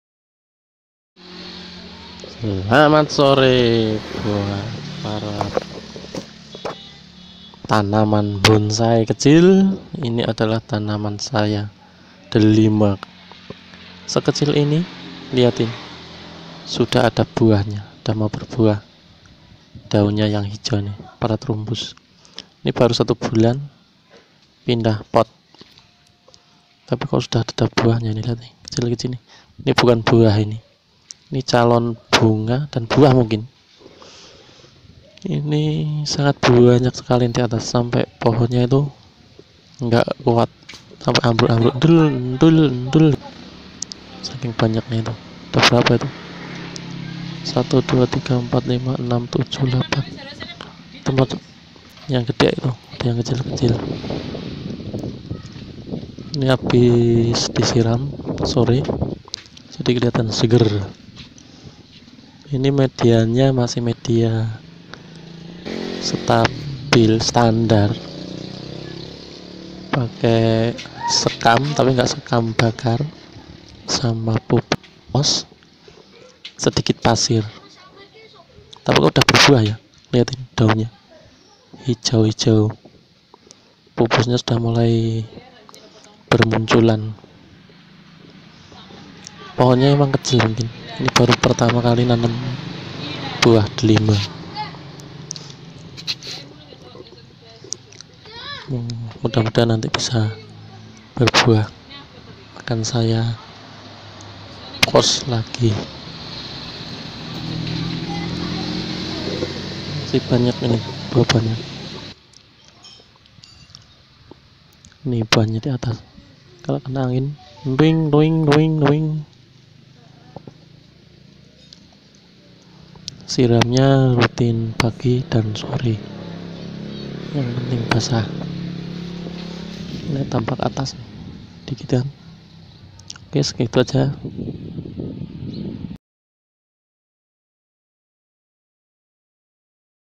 pohon delima!!tabulampot
mencoba menanam tabulampot.
baru tanam satu bulan udah ada tanda tanda bunga.semoga bisa jadi